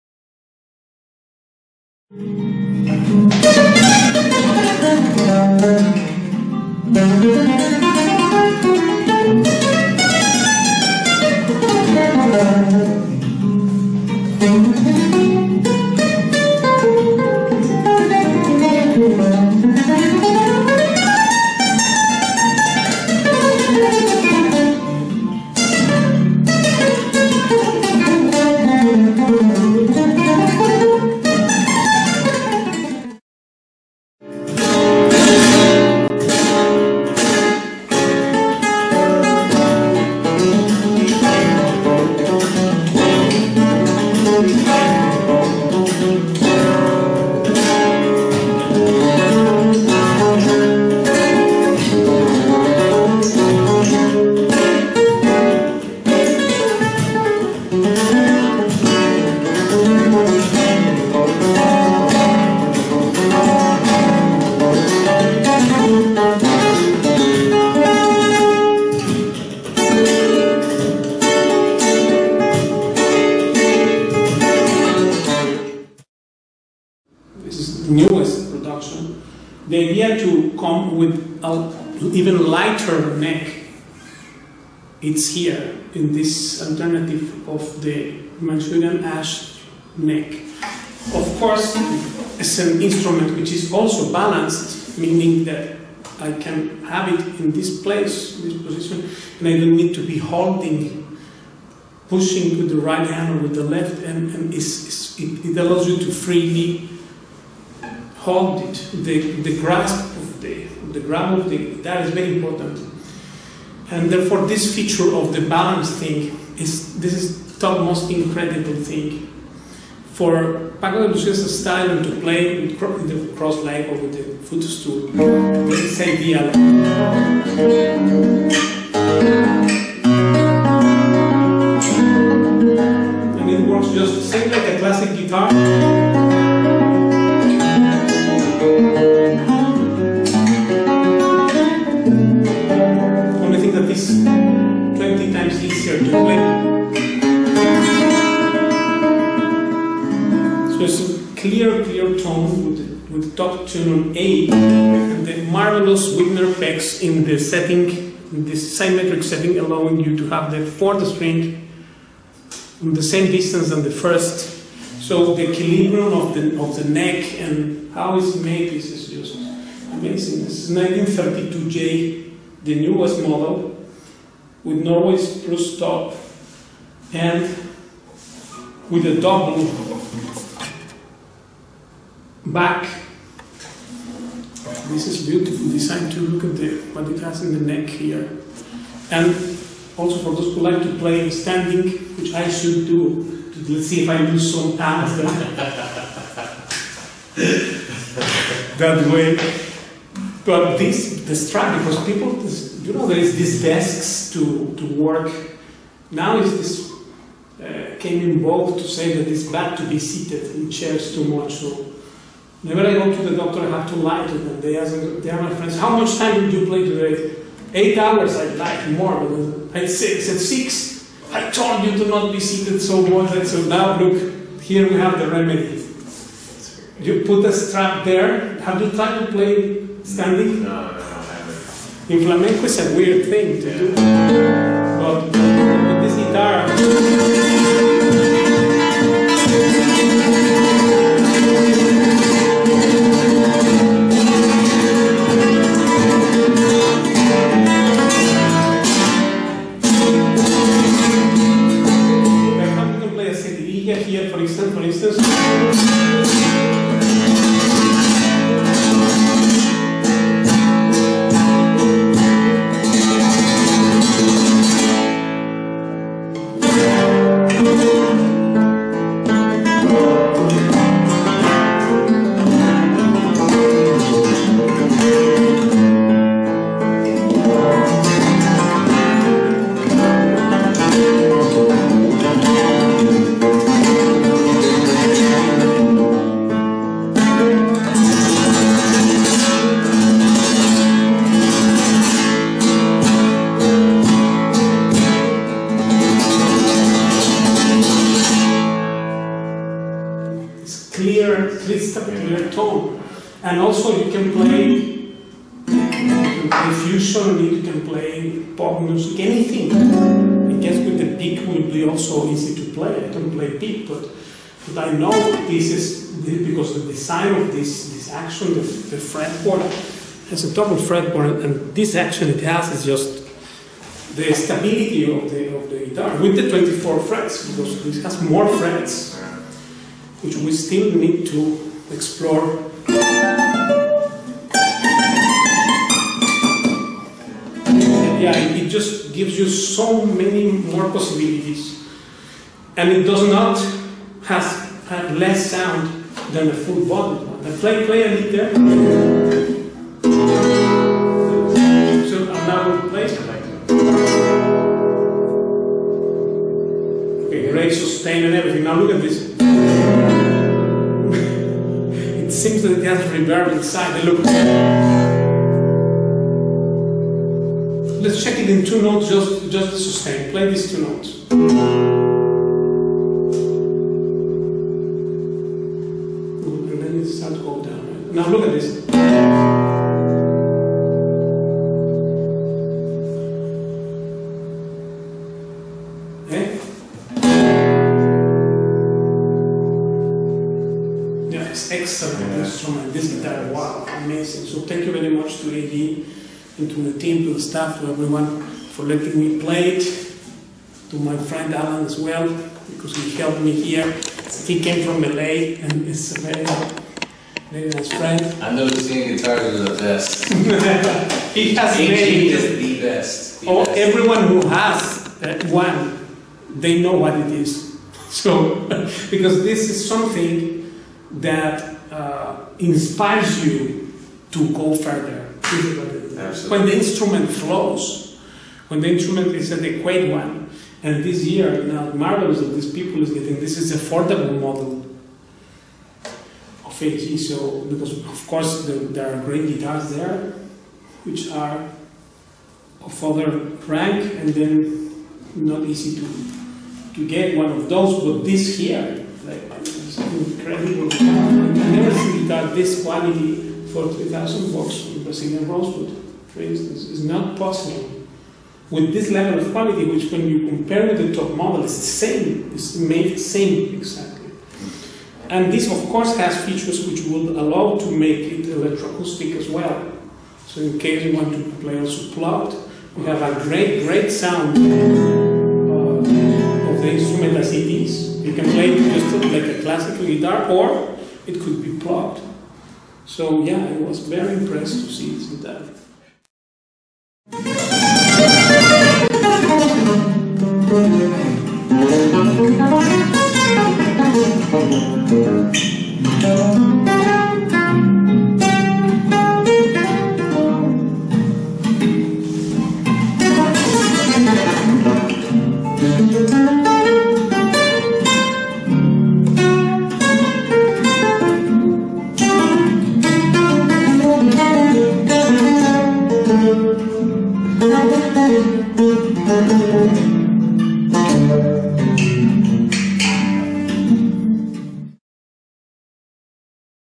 Accessible Balance Rosewood cut away Wittner Pegs 24 frets Simplicio 1932 j/Andalusian Guitars Spain
References to the history of the double sound hole on the top
Simplicio noticed a disadvantage of the traditional sound hole position in the weakening of the guitar top in a crucial area and also in a reduction of the sound emitting surface of the top (Huber, J.: The Development of the Modern Guitar, Westport, 1994, p.27). The increased sound emitting surface should also improve the sound emitting in the bass range (Stark, D.: Diplomarbeit – Entwurf und Bau einer Konzertgitarre mit doppelter Decke in Sandwichbauweise, Weimar, 2006, p.6)

Impact on the sound:

Omitting the sound hole or positioning it on both sides of the fingerboard leads to an enlarged vibrating surface of the top. Cue to the ratio between top length or width and top thickness, it is possible to:

– attain the same degree of flexibility of the top as in a traditional guitar, but obtaining a higher mass of the top (this leads to a better sustain without the loss of attack and responsiveness)

– attain a more flexible top than on a traditional guitar, but maintaining the same mass of the top (this leads to increased loudness and responsiveness without the loss of sustain).

can be lowered significantly, which in turn has an impact on the tonal character of the instrument.

Affordable red cedar top double cut away Wittner Pegs 24 frets Simplicio 1932 fe /Andalusian Guitars